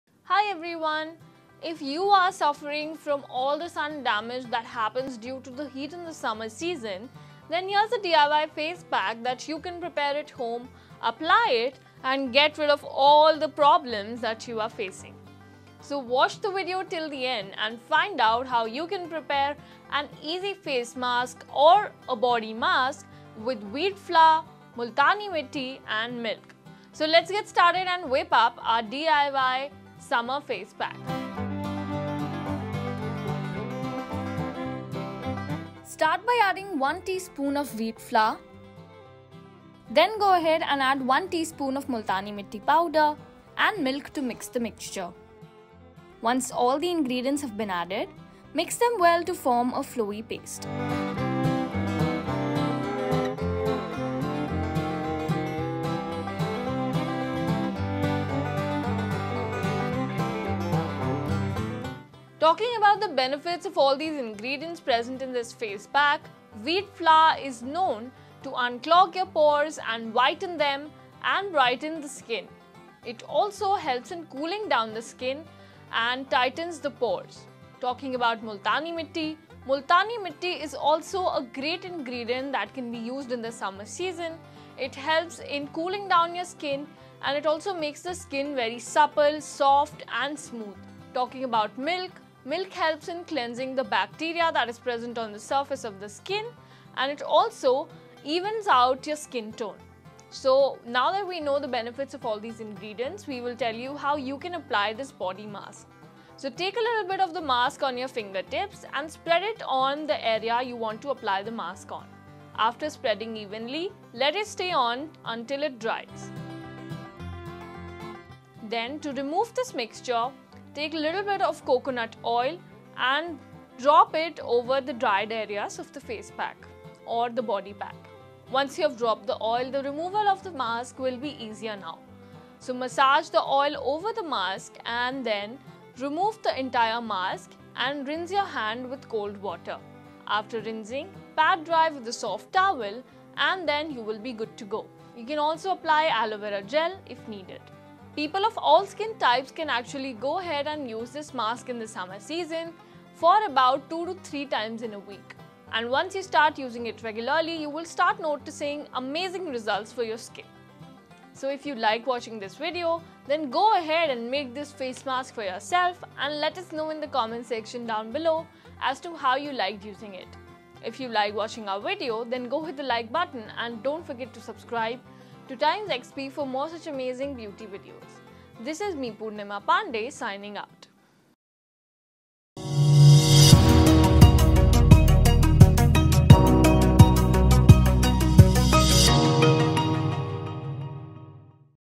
How To Get Rid Of Sun Damage | Wheat Flour Face Mask | TimesXP Beauty
Wheat flour is also extremely beneficial for the skin and if you too want to add this ingredient to your beauty regime then here is a DIY recipe for you to check out.